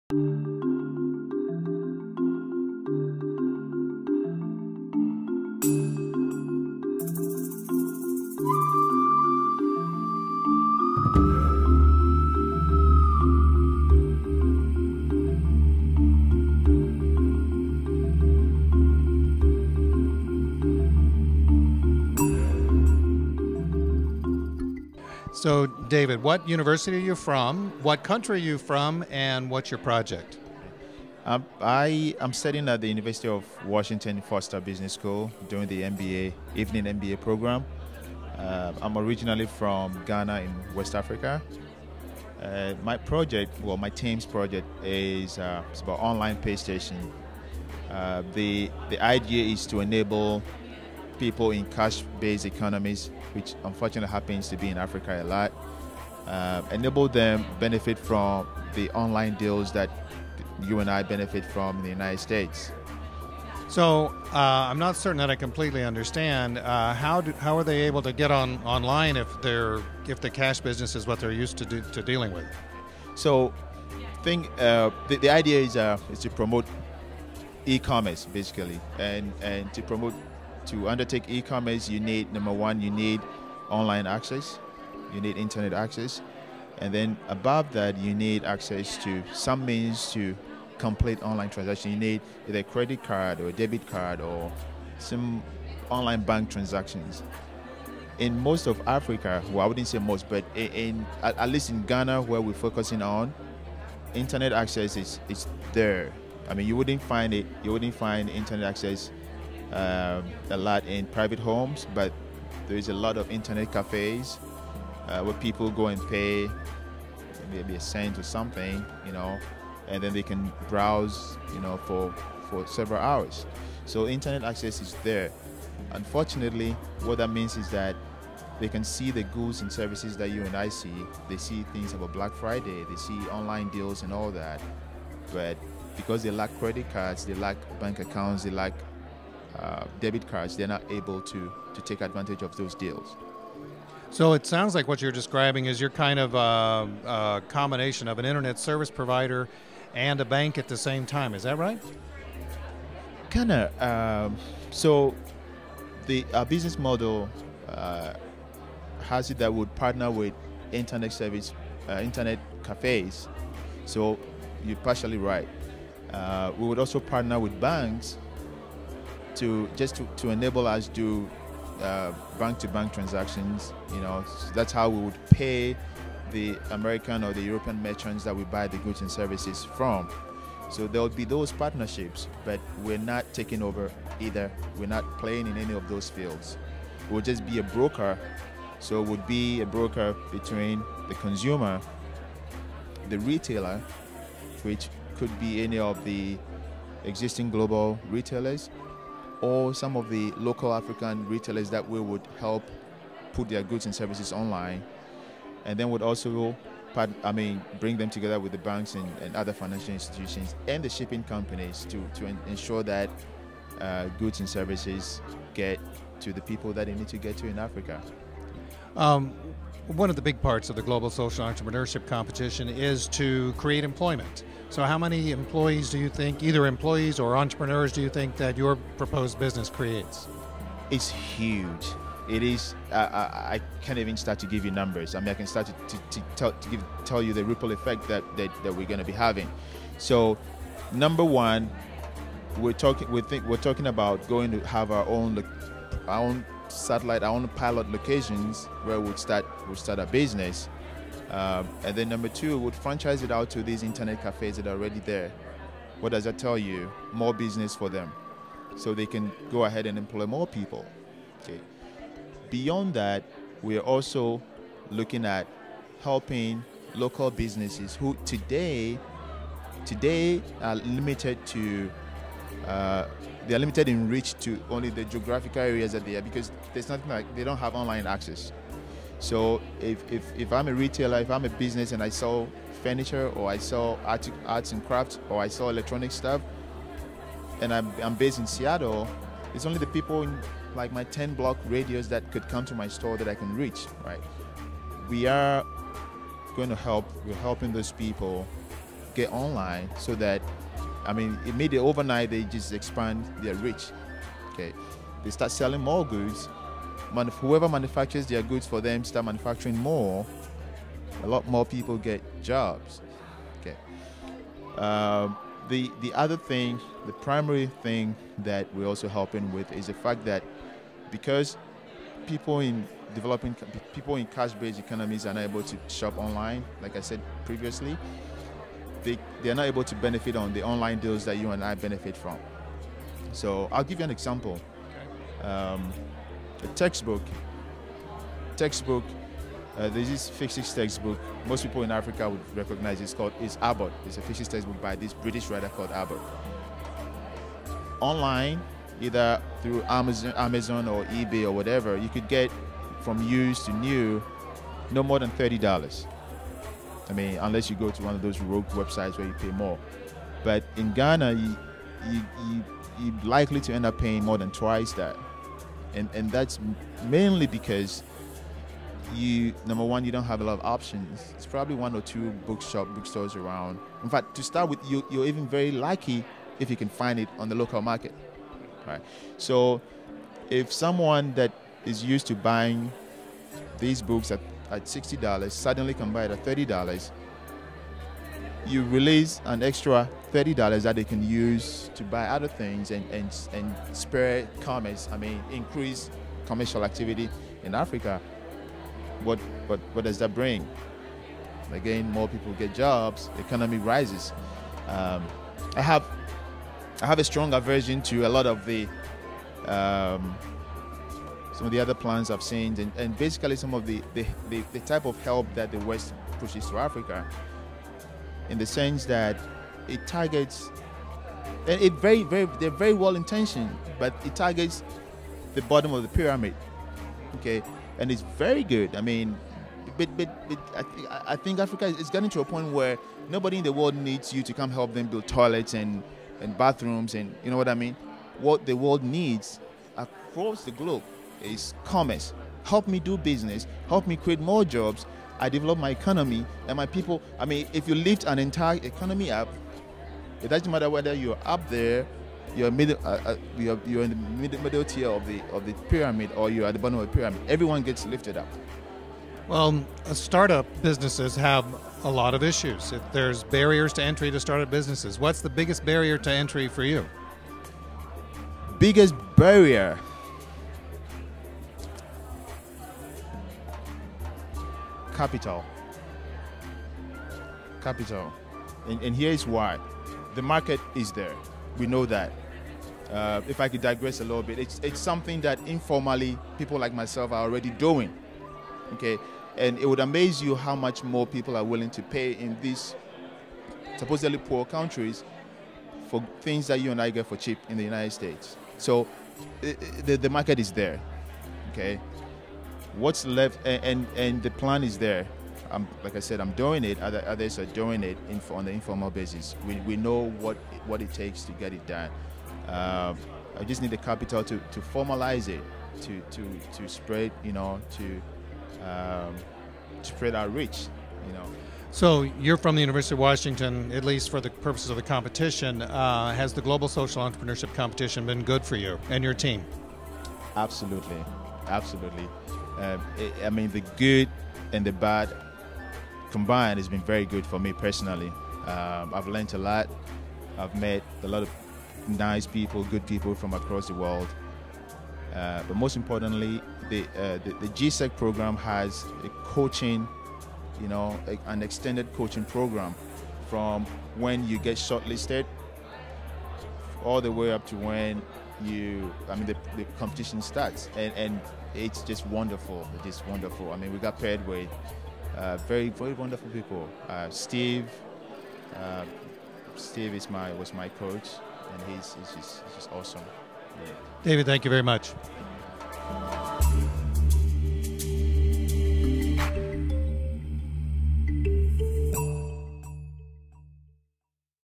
David Anna, Online PayStation (GSEC 2011)
Online PayStation: To extend the benefit of online bargain shopping to the world's poor by accepting cash. Our vision is to increase the accessibility and purchasing power of low income communities worldwide by establishing a network of pay stations that will provide savings on and online access to items such as school books, food, medicines, and water.

GUEST: David Anna, University of Washington, Seattle, WA, USA